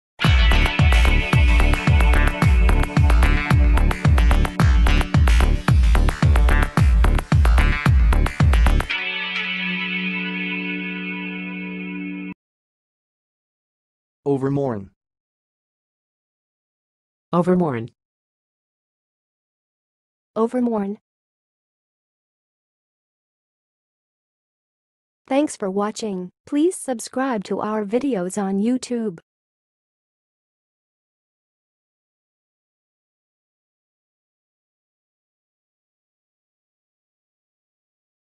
How to say "overmourn"! (High Quality Voices)
Watch in this video how to say and pronounce "overmourn"!